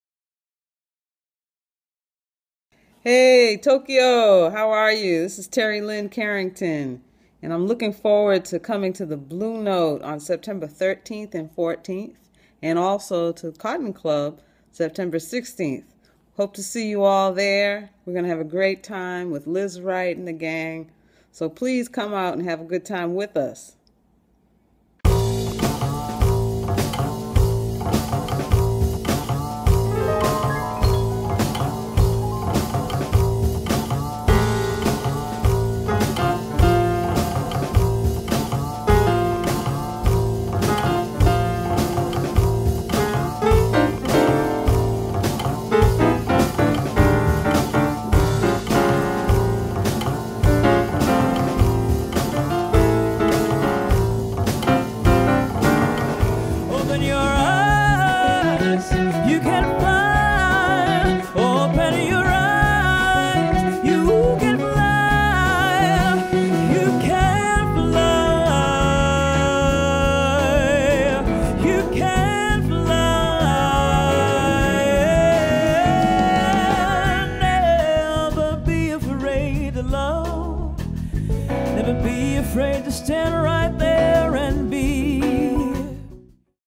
[Message+Trailer] TERRI LYNE CARRINGTON featuring LIZZ WRIGHT : COTTON CLUB JAPAN 2014 trailer
From Terri Lyne Carrington (ds)

グラミー賞常連のトップ・ドラマー
人気シンガーを迎えての来日が急遽決定

TERRI LYNE CARRINGTON
featuring LIZZ WRIGHT
テリ・リン・キャリントン
フィーチャリング・リズ・ライト

MEMBER
Terri Lyne Carrington (ds), Lizz Wright (vo), Peter Martin (p,key),
Tia Fuller (sax), Zack Brown (b), その他メンバー未定

MUSIC CHARGE
￥8,500